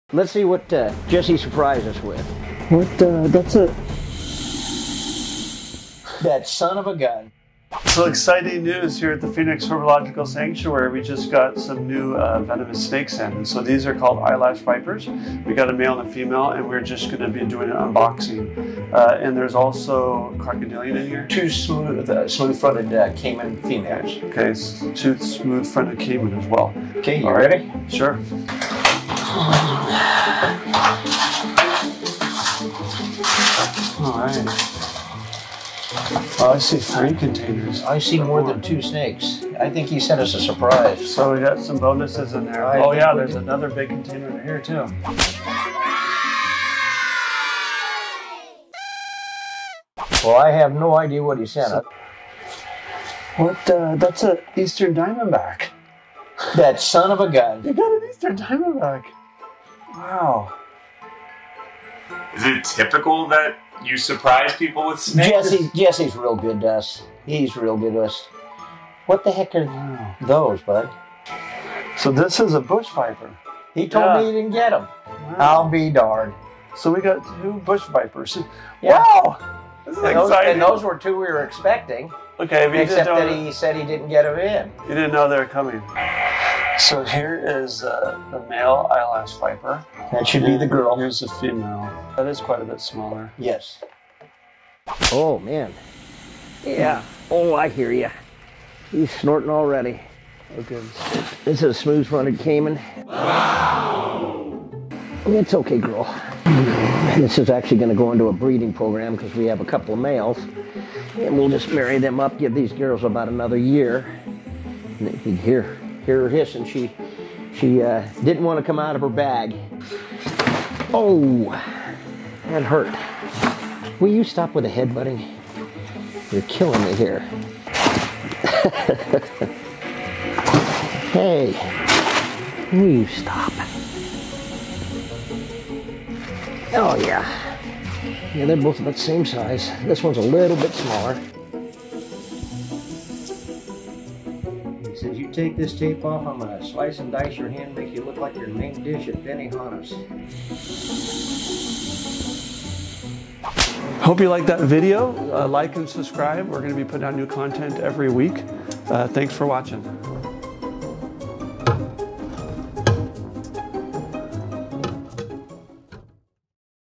SURPRISE REPTILE UNBOXING (Dwarf Caiman, Eyelash Vipers, and more!)
A new shipment came from another zoological facility last week with some animals we were expecting (and some CRAZY surprises.) Russ and Cale prepare to unbox these animals so they can be transferred into quarantine.
In the end, we received 2 west African bush vipers, a pair of eyelash vipers, an eastern diamondback rattlesnake, and two smooth-fronted dwarf caimans!
All of these animals are out an on exhibit now for you to see!